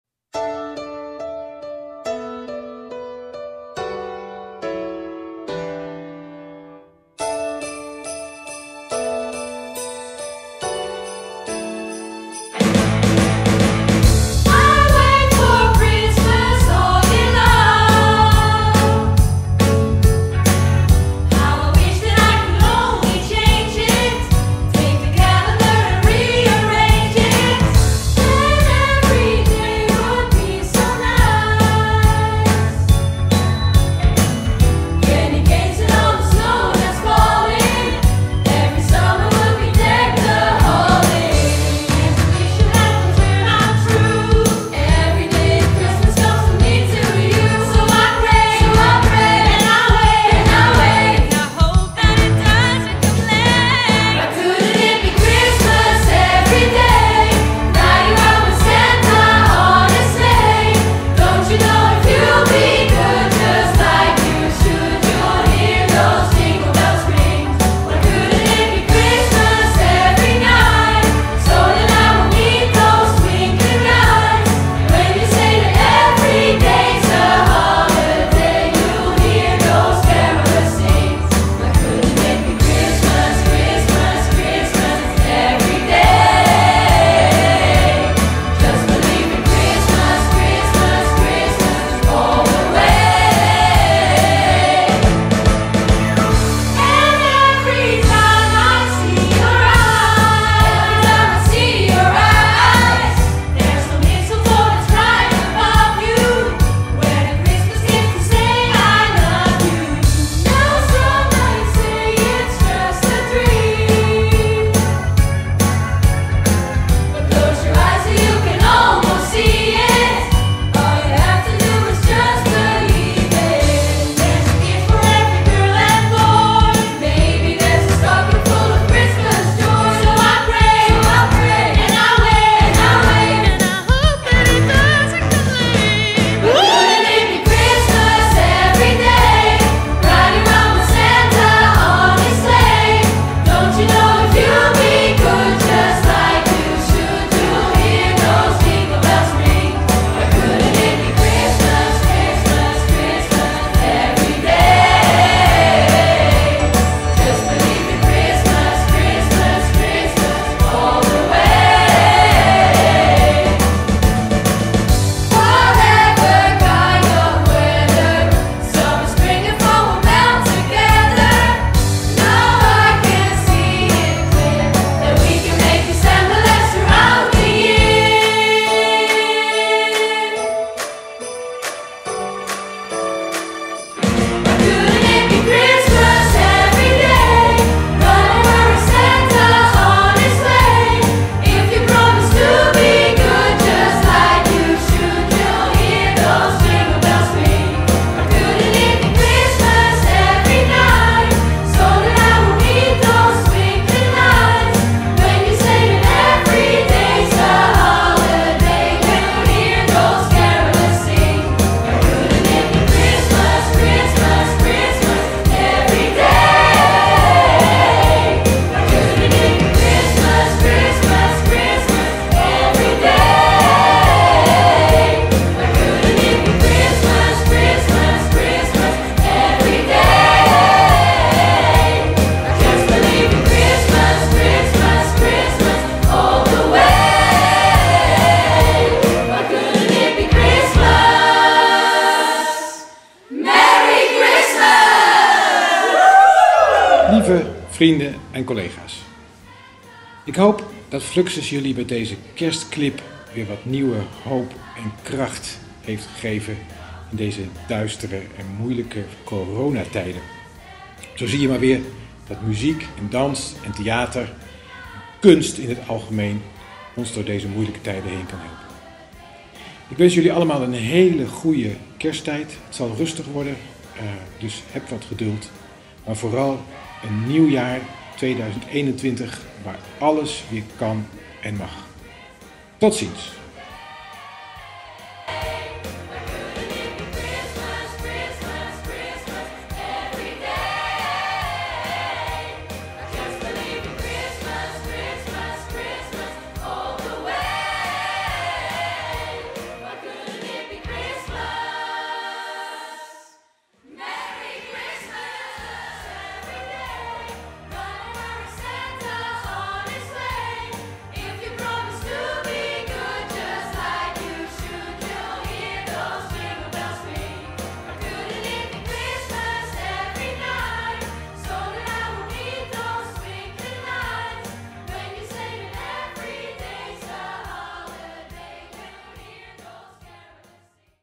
Kerstgroet FluXus 2020
Met deze video - gemaakt door de FluXus Musical Academy - wensen we je namens heel FluXus fijne feestdagen en een gezond, veilig en creatief 2021!